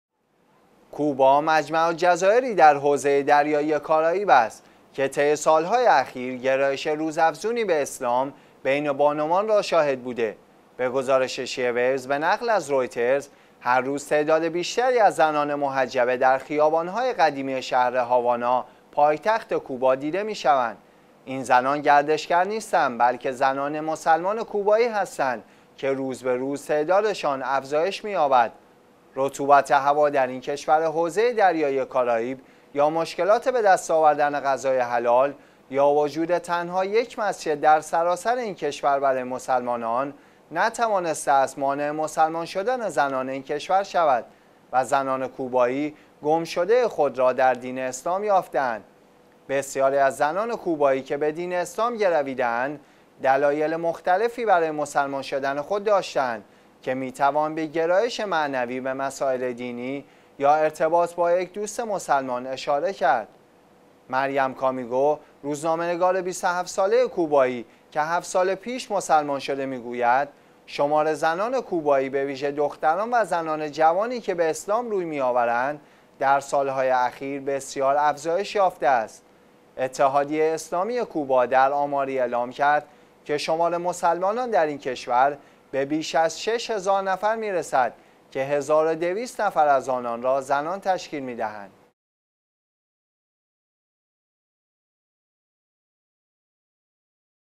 گرایش روزافزون زنان به اسلام در کوبا
کوبا مجمع‌الجزایری در حوزه دریای کارائیب است که طی سال‌های اخیر گرایش روز افزونی به اسلام ، بین بانوان را شاهد بوده.
به گزارش شیعه ویوز به نقل از رویترز، هر روز تعداد بیشتری از زنان محجبه در خیابان‌های قدیمی شهر «هاوانا»، پایتخت کوبا، دیده شوند؛ این زنان گردشگر نیستند بلکه زنان مسلمان کوبایی هستند که روزبه‌روز تعدادشان افزایش می‌یابد.
رطوبت هوا در این کشور حوزه دریای کارائیب یا مشکلات به‌ دست آوردن غذای حلال یا وجود تنها یک مسجد در سراسر این کشور برای مسلمانان، نتوانسته است مانع مسلمان شدن زنان این کشور شود و زنان کوبایی گمشده خود را در دین اسلام یافته‌اند.
بسیاری از زنان کوبایی که به دین اسلام گرویده‌اند، دلایل مختلفی برای مسلمان شدن خود داشته‌اند که می‌توان به گرایش معنوی به مسائل دینی یا ارتباط با یک دوست مسلمان اشاره کرد.
«مریم کامیگو»، روزنامه‌نگار ۲۷ ساله کوبایی که هفت سال پیش مسلمان شده می‌گوید: شمار زنان کوبایی، به‌ ویژه دختران و زنان جوانی که به اسلام روی می‌آورند، در سال‌های اخیر بسیار افزایش یافته‌است.
اتحادیه اسلامی کوبا در آماری اعلام کرد که شمار مسلمانان در این کشور به بیش از ۶ هزار نفر می‌رسد که هزار و ۲۰۰ نفر از آنان را زنان تشکیل می‌دهند.